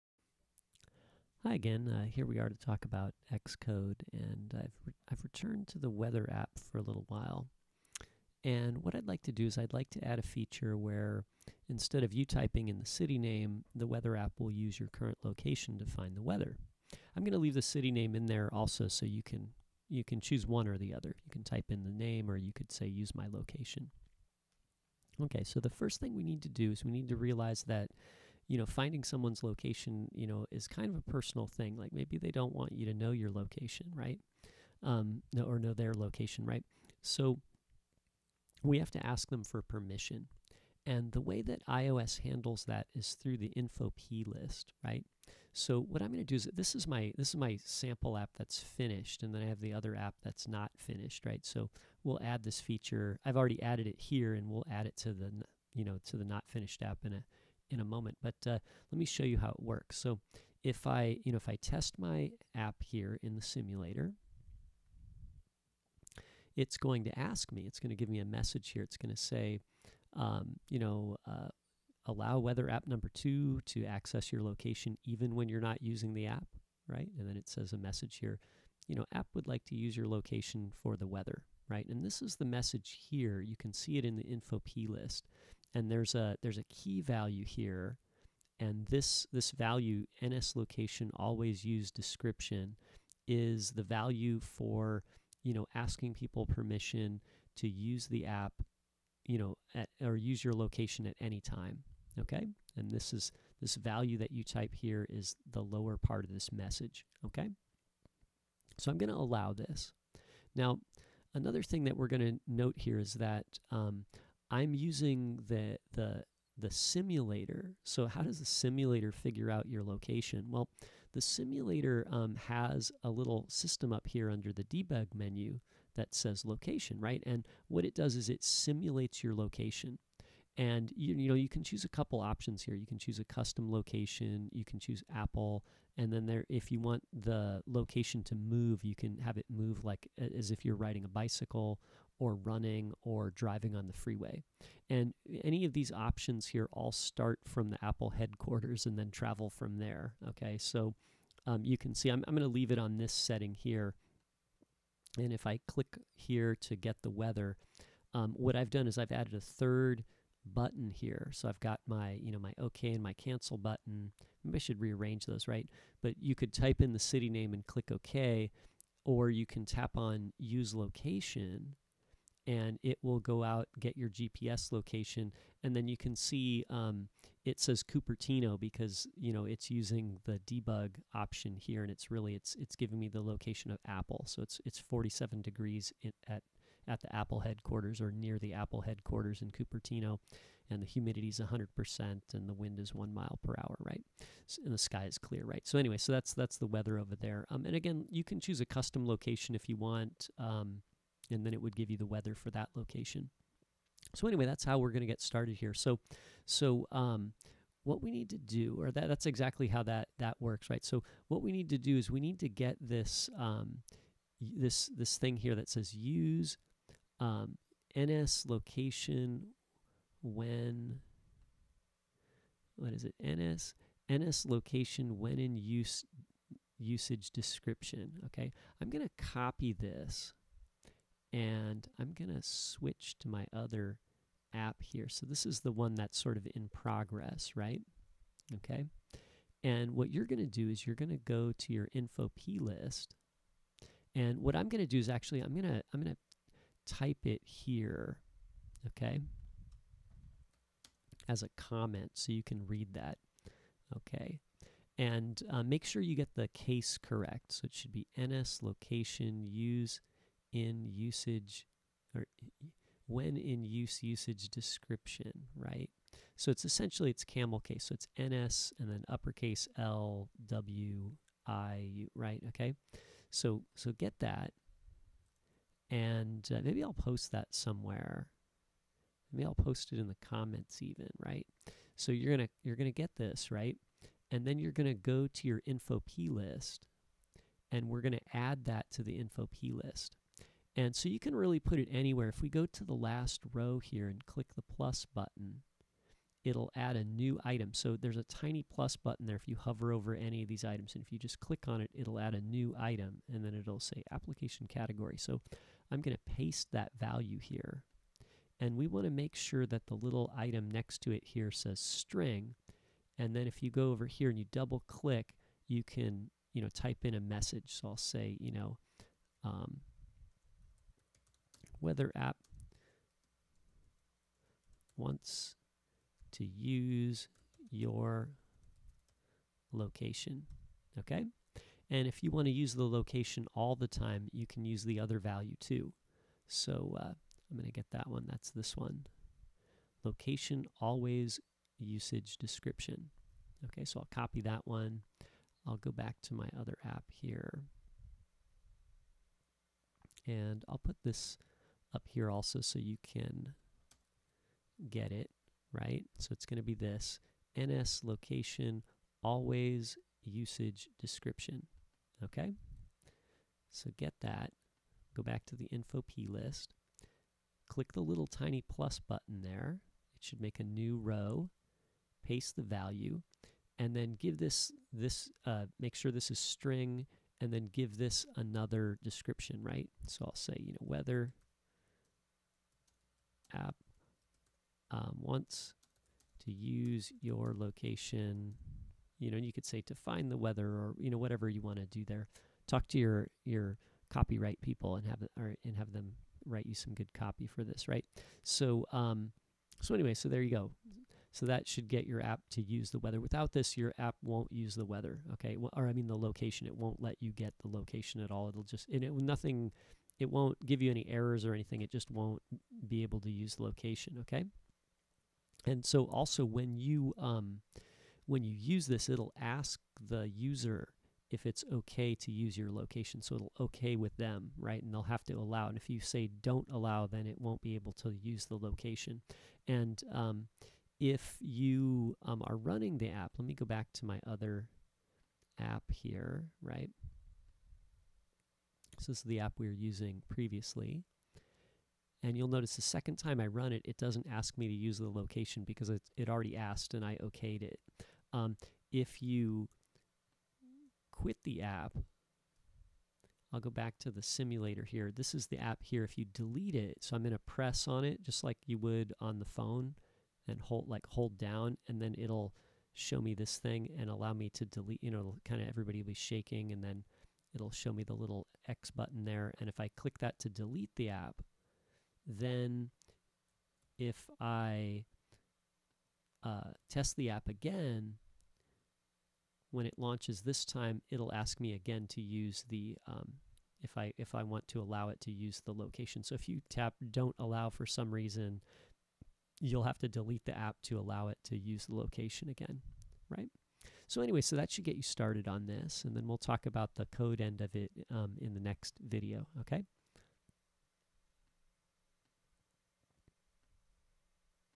20 Weather App Xcode 7 GPS Location info plist
Xcode 7 Getting started with Location services and GPS on iOS. The first step here is setting some options in the info.plist. This enables Location services in your app, and allows you to set the message that is displayed when the app asks permission to use your location.

NSLocationAlwaysUsageDescription
NSLocationWhenInUseUsageDescription